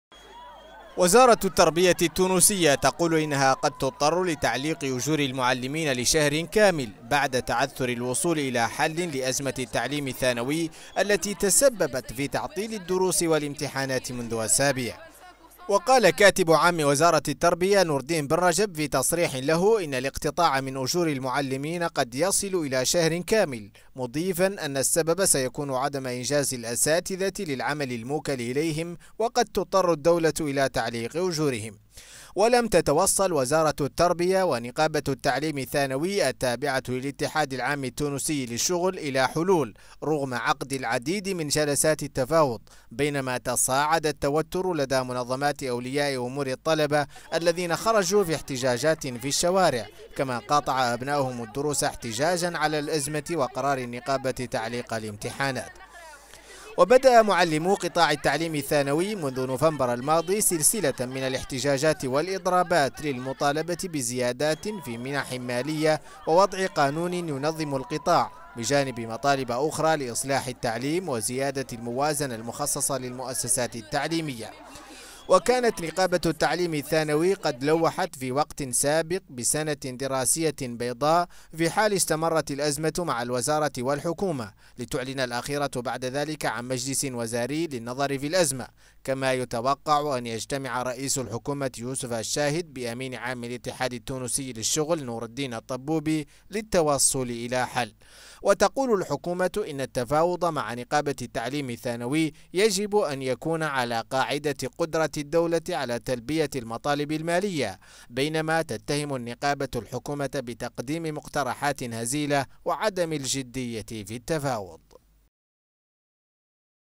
وزارة التربية التونسية تهدد بتعليق أجور المدرسين بعد تعثر التوصل لحل تقرير محمد الأمين امعييف.
هددت وزارة التربية التونسية بتعليق أجور المدرسين، بعد ما تعثر التوصل إلى حل لأزمة التعليم الثانوي، الذي تسبب في إضراب يستمر منذ أسابيع.
وكانت وزارة التعليم التونسية قد عقدت عددا من جلسات التفاوض مع نقابة التعليم الثانوي التابعة للاحاد العام التونسي للشغل، دون التوصل إلى حل.
تقرير: محمد الأمين ولد امعييف.